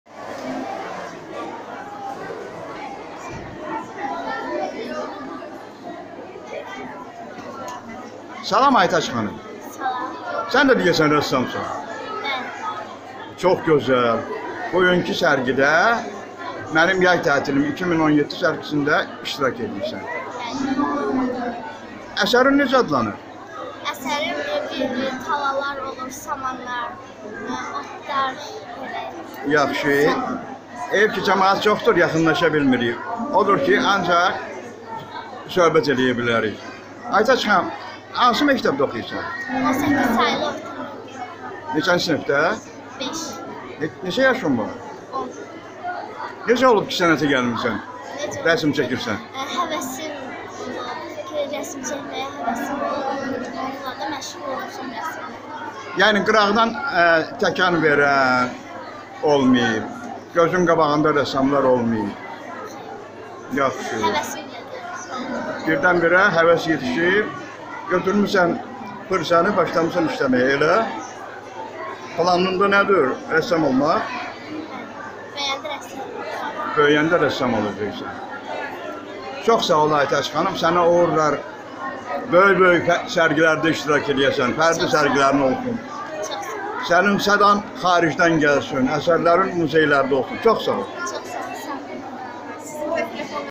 Gənc rəssam Aytac Əsgərovanın jurnalına müsahibəsi
Xətai sənət mərkəzində "Mənim yay tətilim-2017" rəsm müsabiqəsinin yekun sərgisi. Gənc rəssam Aytac Əsgərovanın jurnalına müsahibəsi